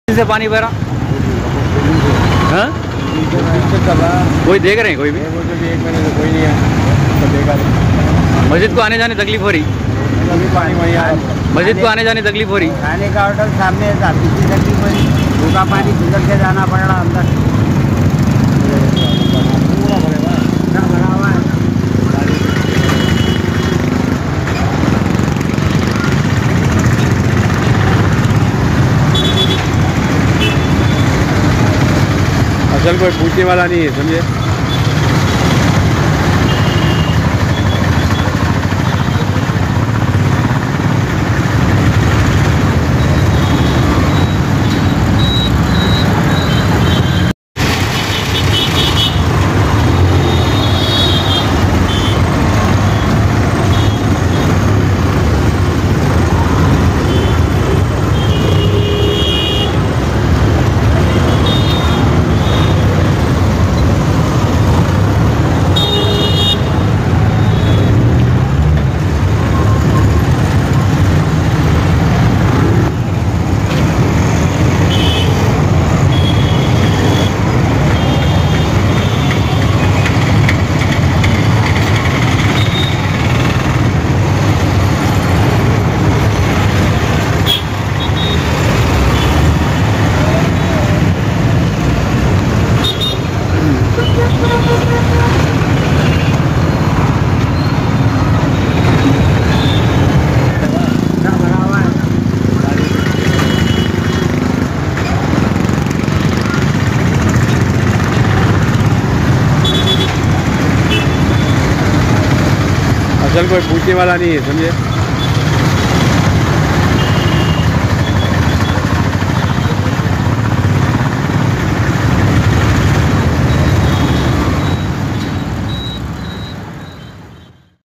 Tadbun X Road Masjid Ke Musali'Yon Ko Ho Rahi Hai Pareshani HMWS&SB Ki Laparwahi Bahadur Pura Se
HMWS&SB Ki Laparwahi| Masjid Ke Musali'Yon Ko Ho Rahi Hai Pareshani | Tadbun X Rd| Hyderabad | Telangana

Ji Haan Nazreen Khabar Hai Purane Shaher Ke Ilaqe Tadban X Rd Bahadur Pura Se Jahan Par Pichle Ek Mahine Se Ganda Pani Road Par Beh Raha Hai Hyderabad Metropolitan Water Supply And Sewerage Board Ki Khuli Laparwahi Dekhi Jaa Sakti Hai Ye Pani Ki Wajha Se Masjid Ko Jaa Rahe Musali'Yon Ko Mushkilat Pesh Aarahi Hai

Aaye Dekhte Hai Aage Ki Tafseelat